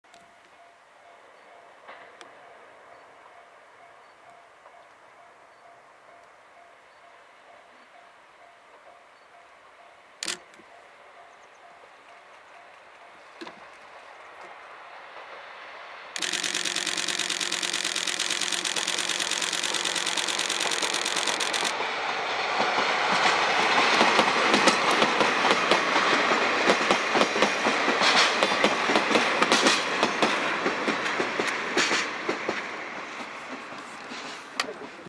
DD51 プッシュプル 八高線 試運転 DD51-842+旧型客車+DD51-895
2010/12/5　八高線　群馬藤岡～丹荘　　試9220レ　DD51-842+旧型客車+DD51-895